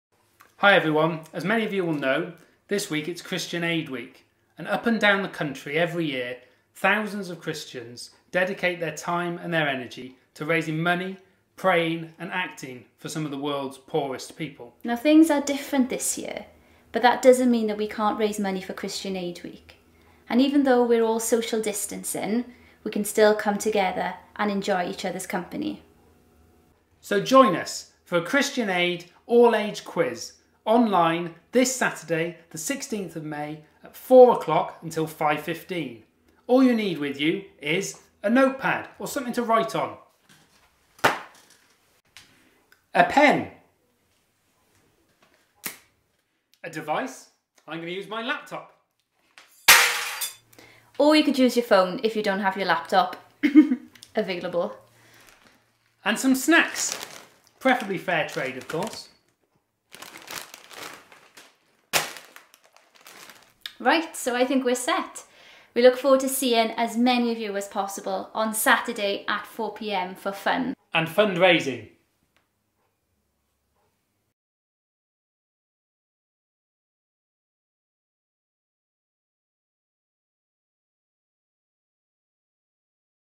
Christian Aid Week Quiz Promo Video
Watch this to find out about the Christian Aid Week Quiz online with Pete and Beth on Saturday 16 May 2020 at 4pm. It's on zoom but you can choose to join the quiz with or without your video switched on.